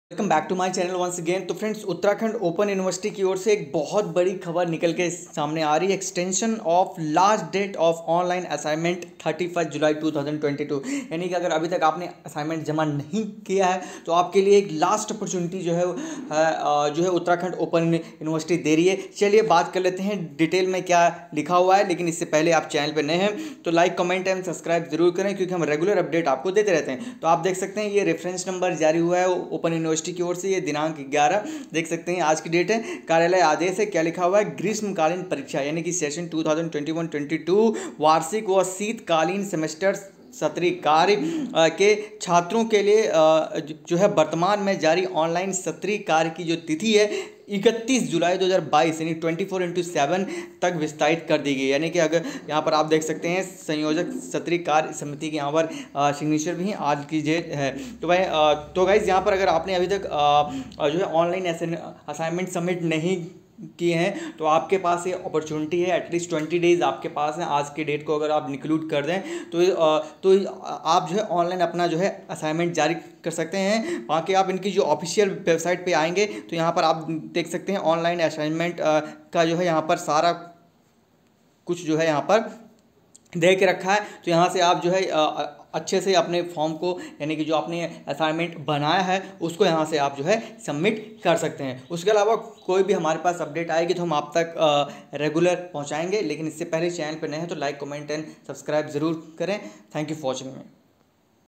UOU: Notice: Extension of last date of Online Assignment till 31 July 2022
uttarakhand open university date sheet
uttarakhand open university exam
uou back paper date sheet